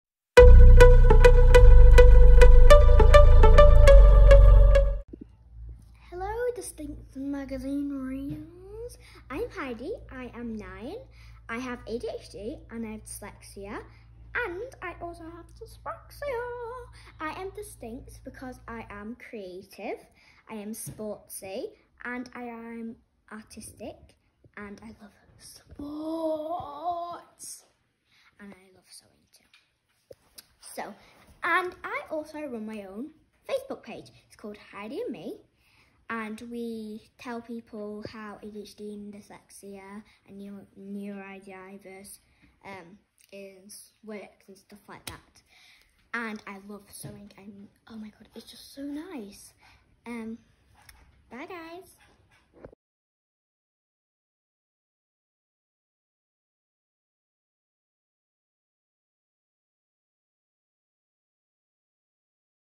Heidi Parody message for dyslexics
9-year-old Heidi Parody shares her personal experiences with the world to challenge the stigma around neurodiversity and empower neurodivergent kids and their carers to embrace their difference.  Get in touch Heidi: Heidi and Me. Our Neurodiversity Journey.

Read more on Heidi and her journey on the September 2021 issue of the Dystinct Magazine.

Get in touch with Heidi